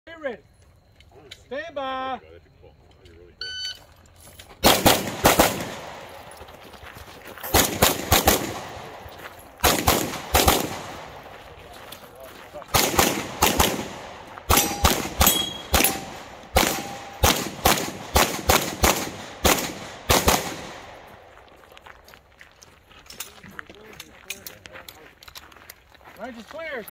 2021 Open Nationals Stage 18
2021 Open Nationals stage 18.

This was my last stage of the match and one of my favorite stages.

I really enjoy the stages that your scores are a direct reflection of your effort as well as your skill.

This stage was all about effort until you get to the end, then the shooting skills took over.

Tcsacoaching.com